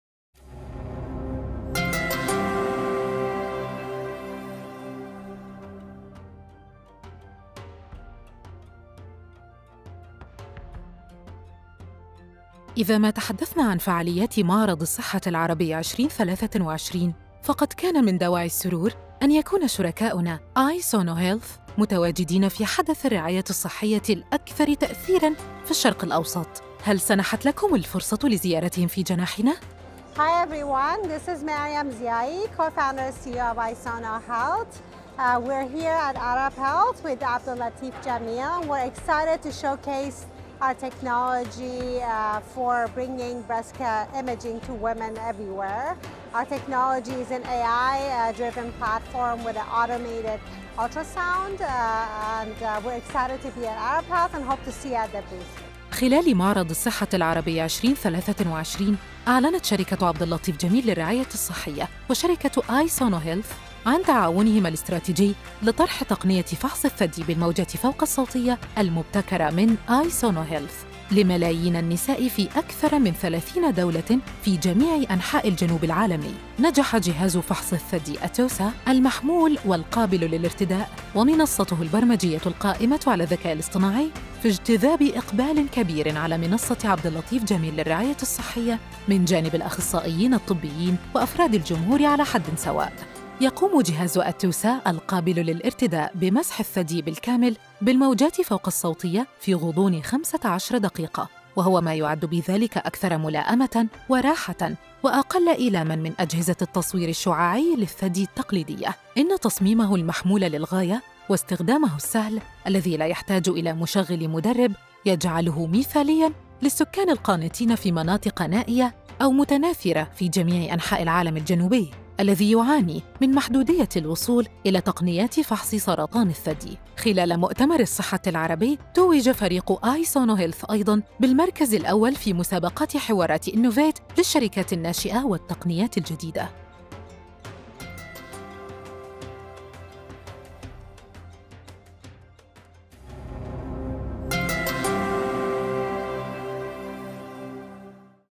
عبد اللطيف جميل للرعاية الصحية وآي سونو هيلث iSono Health
جاء الإعلان عن الشراكة بين شركة عبد اللطيف جميل للرعاية الصحية وآي سونو هيلث iSono Health  في اليوم الأول من معرض الصحة العربي 2023، ستعمل عبد اللطيف جميل للرعاية الصحية كموزع حصري في الجنوب العالمي لنظام “أتوسا” (ATUSA) الذي ابتكرته شركة “آي سونو هيلث” (iSono Health)، وذلك في سبيل إتاحة الجهاز بصفة مبدئية لمئات الملايين من النساء في 31 دولة في منطقة الشرق الأوسط وشمال إفريقيا وأفريقيا وجنوب آسيا وجنوب شرق آسيا.

 في الفيديو التالي تحدثنا الدكتورة مريم ضيائي المؤسس الشريك والرئيس التنفيذي لشركة آي سونو عن هذه الشراكة.

As we look back fondly at our success and that of our partners at Arab Health 2023, we hope you'll enjoy these video shorts.

In this one we hear from Dr. Maryam Ziaei, Co-Founder and CEO of iSono Health.